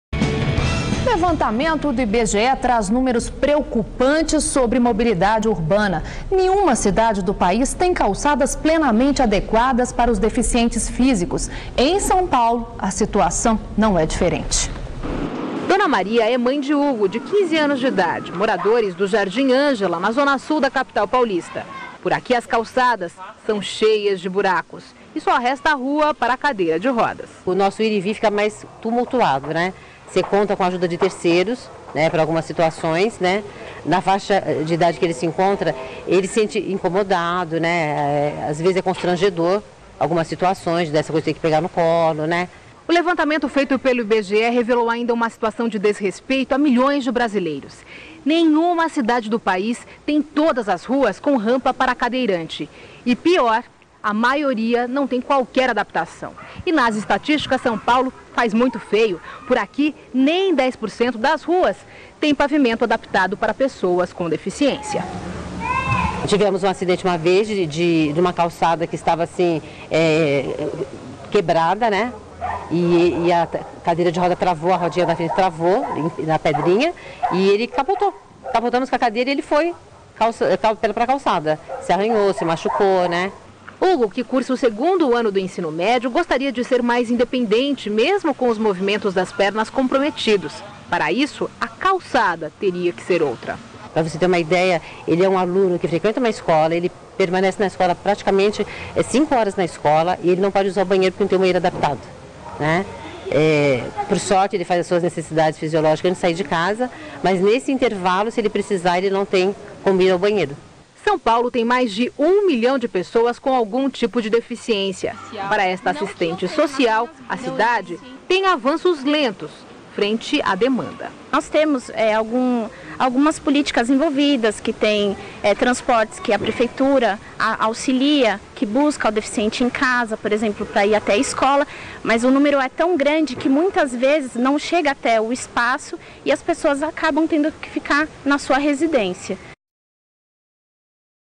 Faltam rampas para os cadeirantes
Levantamento do Ibge traz números preocupantes sobre mobilidade urbana: nenhuma cidade do país tem calçadas plenamente adequadas para os deficientes físicos. Em São Paulo a situação não é diferente.

Tema: Cadeirantes